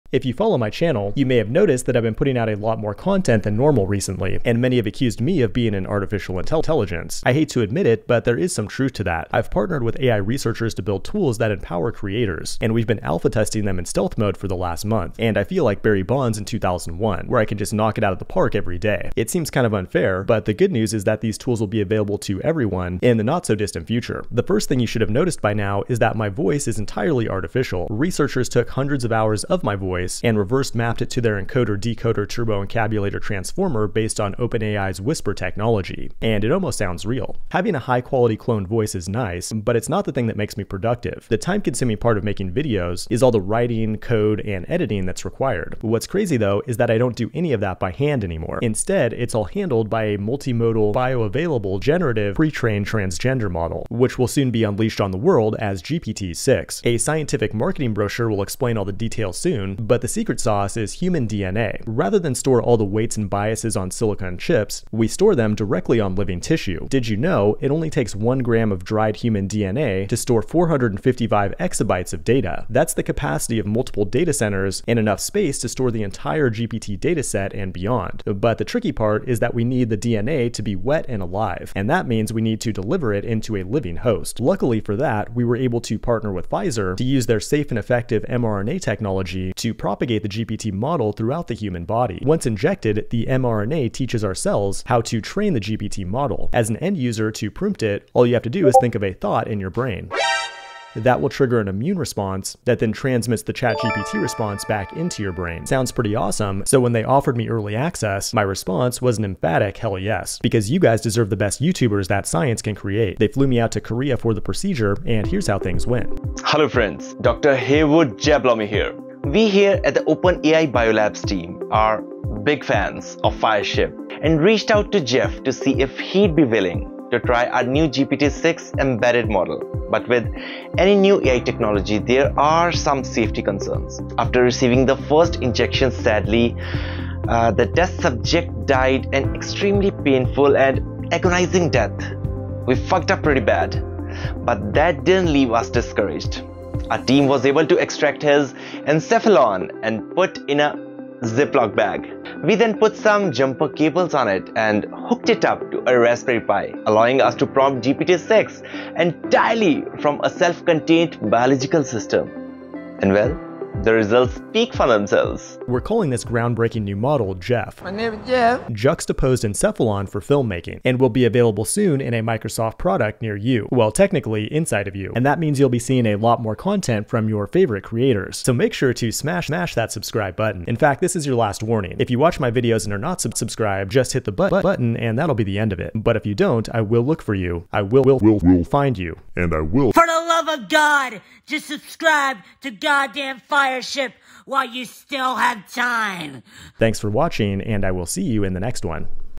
AI is giving me an unfair advantage
I share my secret to gaining 10x productivity with bioavailable artificial intelligence for video production, coding, writing, and more. In case you didn't notice, this is satire.

Cameo by @rickyberwick

🔖 Topics Covered

- Can AI be embedded in the human body?
- Jokes about OpenAI
- What might the future of AI tech look like?